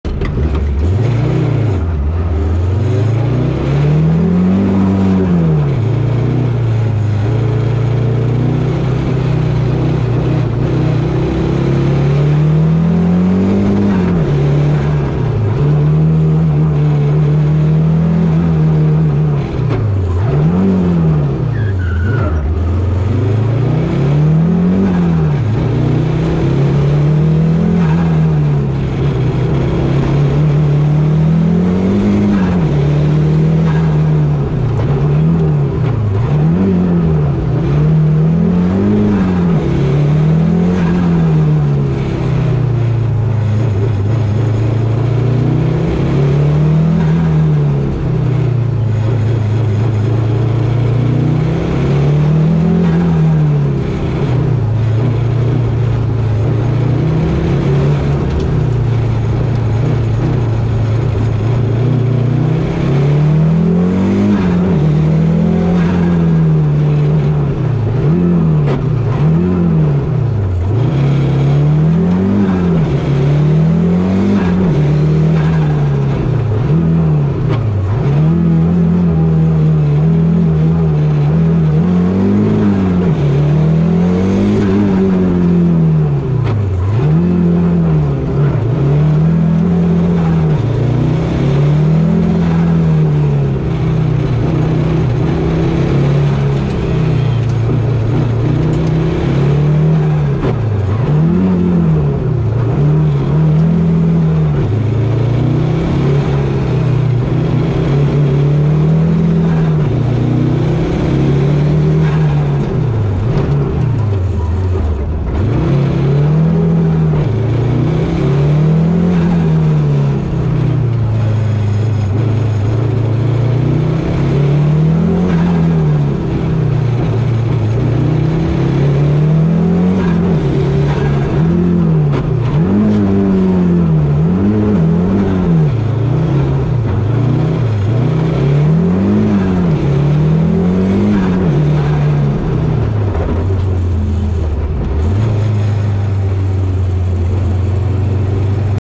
Memoriał 2023 - Onboard | Lenczowski / Wicher - Audi Quattro A2 - OS3
Jesteśmy zespołem rajdowym występującym na odcinkach specjalnych całej Europy.